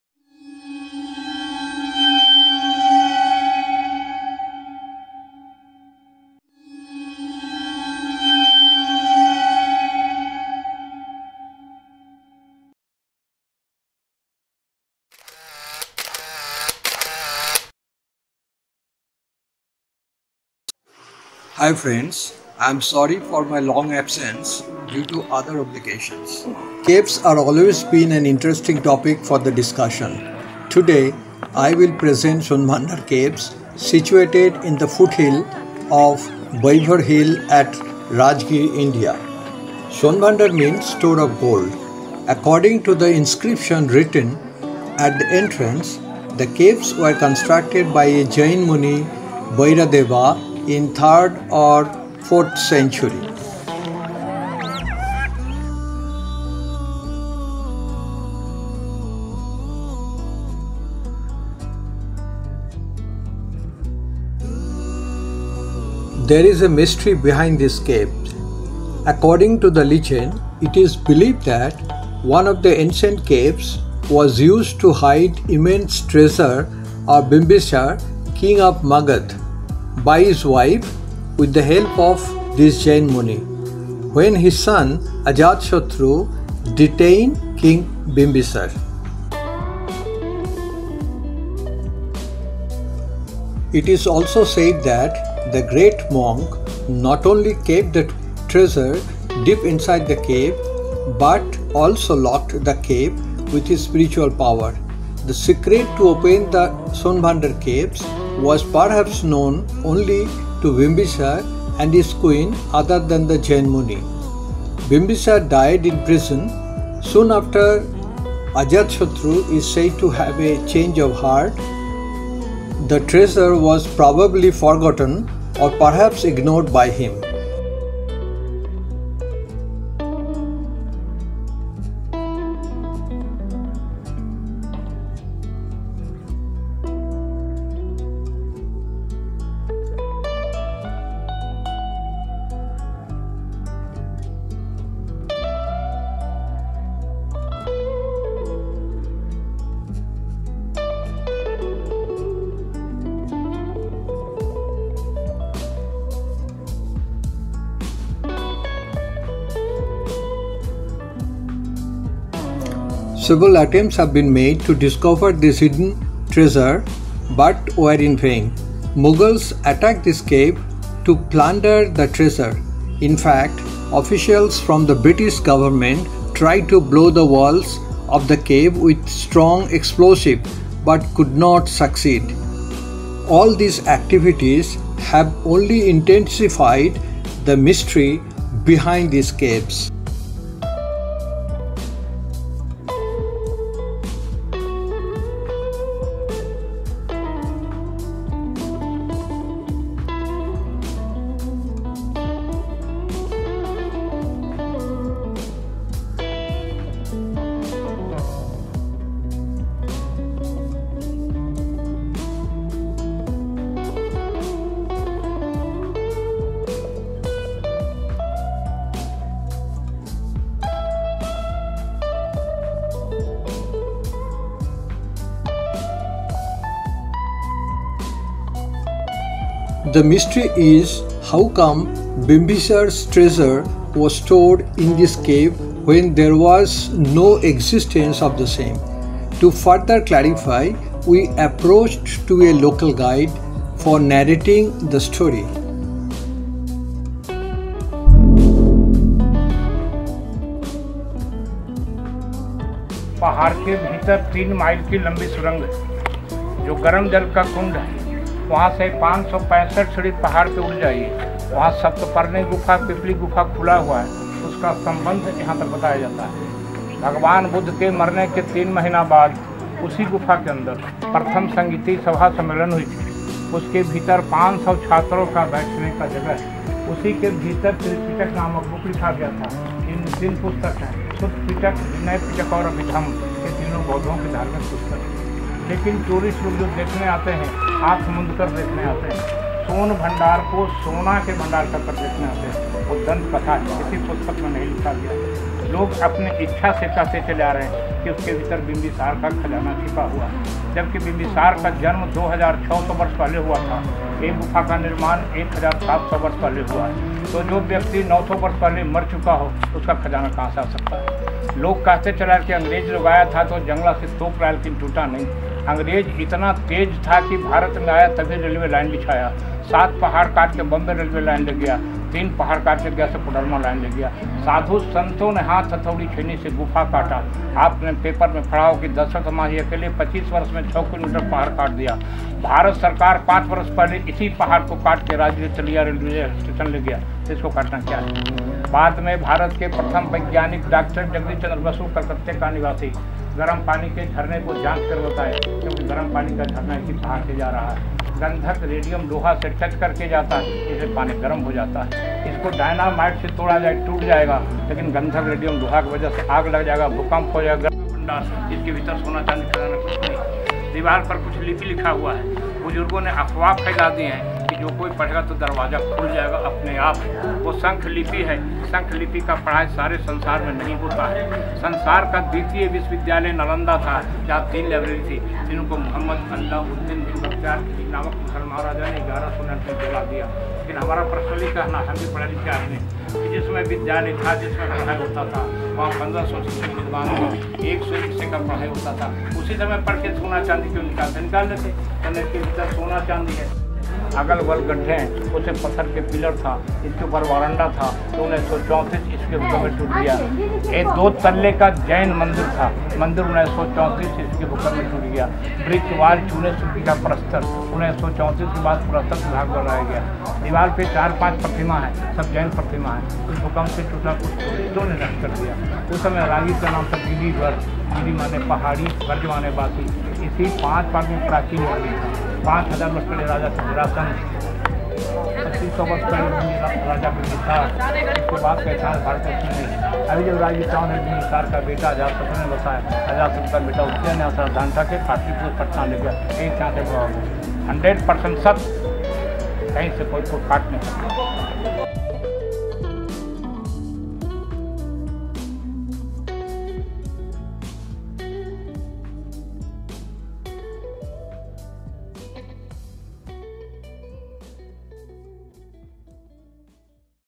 Mystery Always Associate with History: Son Bhandar Caves, Rajgir, India
Caves have always been an interesting topic for the discussion. Owing to the secret tales surrounding them, caves have inevitably managed to capture the attention of traveler, researchers and historians at large. India too has a fair number of Caves that are still unexplored and well-hidden, and one among them is the Son Bhandar Caves in Rajgir, Bihar. Son Bhandar or Sonebhandar caves, basically is a set of two artificial cave structures located in the foothills on the Vaibhar Hills. As per an inscription written in Gupta language on a rock at the entrance, these caves were constructed by a Jain muni, meaning ‘wise man’, named Vairadeva, which makes them structures belonging 3rd from 4th century BC.

Bimbisara was the son of a chieftain. However, through his prowess, he became king only at the age of 15 and established an empire only few can dream of. Through battle, alliance and marriage – King Bimbisara created a huge and powerful kingdom. His capital was Pataliputra (modern day Patna) and his Magadhan empire had expanded far and wide across India bringing him immense power and – wealth.
The great King Bimbisara became well known for many achievements but is most well remembered as the friend and protector of Gautam Buddha. In his older years, Bimbisara had turned his attention away from material conquests and more towards spiritual ones. He was generous towards ascetics and monks. He gave away his wealth open handedly to help the monks who were carrying the spiritual messages of Hindus, Jains and Buddhists.
King Bimbisara’s illustrious and ambitious son, Ajatashatru was a legendary warrior. However, he could not appreciate his father’s compassion towards these monks. Some courtiers added fuel to the fire and the son resolved to relieve the old king from his duties. Bimbisara was usurped by his own son and put into a dark prison.
While Ajatashatru seized the throne, it is believed that his mother the Queen, hid the brilliant and rare riches of the Magadhan Empire here in the Son Bhandar Caves. Why the Queen chose this cave, no one knows. This cave was made by Vairadeva, a wise Jain monk and was used as a place for meditation.
The Queen approached the monk for safekeeping the royal treasure in the two roomed caves. It is said that the great monk had not only kept the treasure deep inside the cave but also locked it with his spiritual powers. That itself is the reason why no one and no machine has been able to pry into it.
Several attempts have been made to find the ‘supposedly’ hidden treasure but in vain. In fact, once officials from the British government tried to blow the walls of the cave with strong explosives but could not succeed. All these happenings have only intensified the mystery around these caves.

Besides, the chambers of this caves are beautifully polished, which add to the beauty and uniqueness of these. There are several inscriptions in and around the caves, which as per belief is left by the pilgrims, who must have visited the place once.

Legend says that the Son Bhandar Caves are locked by a mantra and can only be opened by its own ‘password’. The town of Rajgir, famous for its hot water springs also brings many tourists here. Most are here for sightseeing but there are quite a few who try to ‘break the code’.
Out of the two caves, this one is said to have a hidden entrance to the treasure. As per generations of storytelling, if one could open this door it will take you through a tunnel through the hills. The passage connects to the Saptaparni Caves on the other side.

Here is a video of tourists paying a visit to Sonbhandar Caves and listening to its intriguing history.
The cave room is rectangular with a ceiling as high as 1.5 metre. On its bare wall, there is some carving which many see as the hint of a door. Suspicions arise when one also sees an inscription next to this ‘door’. This inscription is referred as the ‘code’ of the Son Bhandar Caves and it is supposedly telling how to open the door. The mystery thickens when historians say they cannot read or interpret this script fully.